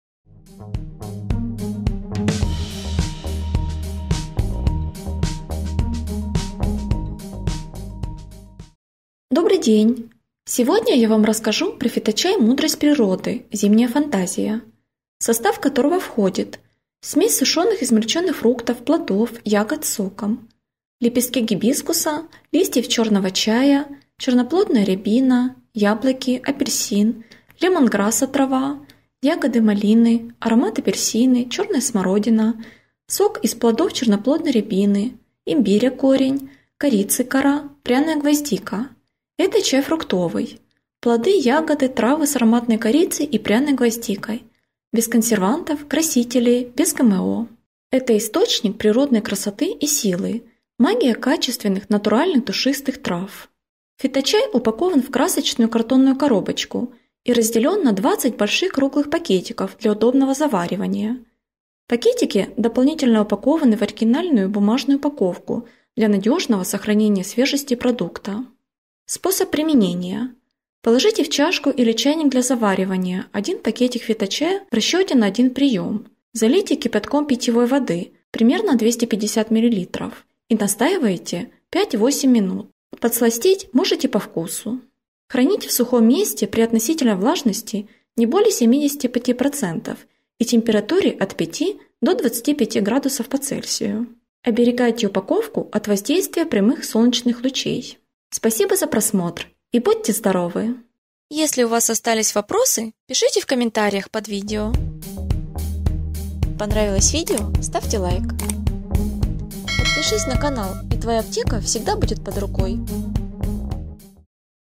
Фиточай Мудрость природы Зимняя фантазия - показания, описание, отзывы
ЗИМОВА ФАНТАЗІЯ
Чай фруктовий. Плоди, ягоди, трави з духмяною корицею та пряною гвоздикою. Без жодних консервантів, барвників, без ГМО. Джерело природної краси та сили. Природньо багатий вітамінами. Натуральна якість. Барвиста картонна коробочка. 20 великих круглих пакетиків чаю для найкращого заварювання. Пакетики додатково запаковані в оригінальну паперову скриньку для надійного збереження свіжості продукту.

Склад: суміш сушених подрібнених фруктів, плодів, ягід з соком: пелюстки гібіскусу, листя чорного чаю, чорноплідна горобина, яблука, апельсин, лемонграсу трава, ягоди малини, аромат апельсин, чорна смородина, сік з плодів чорноплідної горобини, імбиру корінь, кориці кора, пряна гвоздика.

❤️ Внимание!